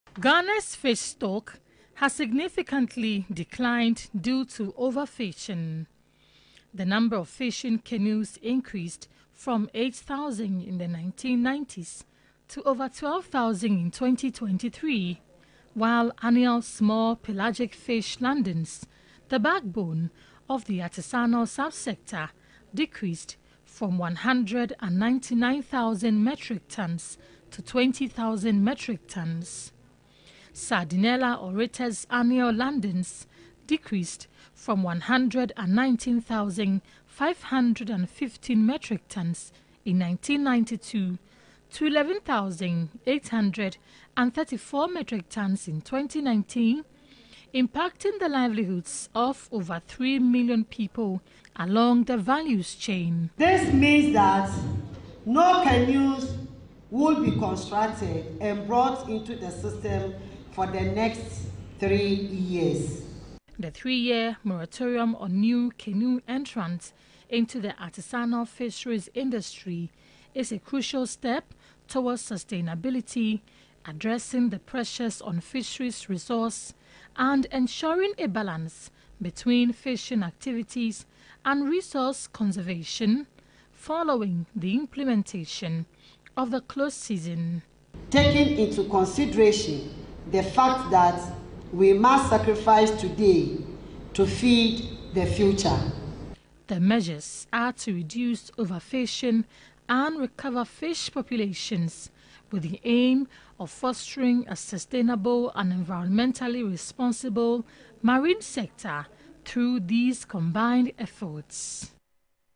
Artisanal fisheries industry: Fisheries Minister announces 3-year moratorium
The Minister of Fisheries and Aquaculture Development, Hawa Komson, has announced a three-year moratorium on new fishing canoes in the artisanal fisheries industry from October 1, following a stakeholder meeting in Accra.

The move is to preserve and sustain Ghana's fisheries resources.